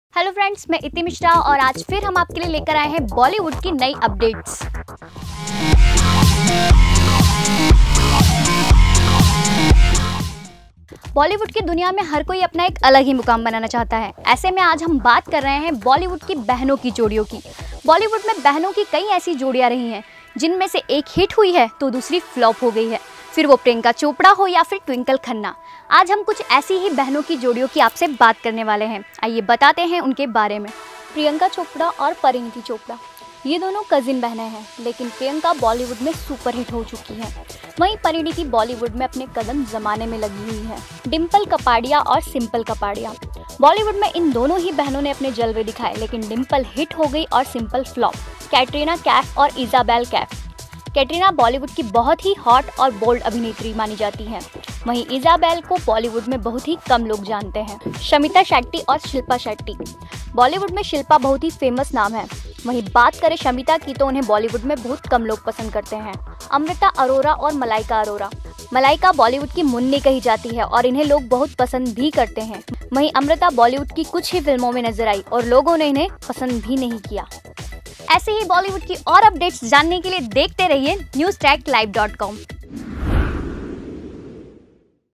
Bollywood Sisters Jodi | Top Hit & Flop Sister Jodi's Of Bollywood
बॉलीवुड में कई ऐसी बहनों की जोड़ियां रहीं है जिनमे से एक फ्लॉप हुई है तो दूजी हिट हुई है. इस लिस्ट में कई नाम शामिल है जो आज हम आपको बताने जा रहें हैं. जी हाँ, और आज हम उन्ही की बातें करने जा रहें हैं.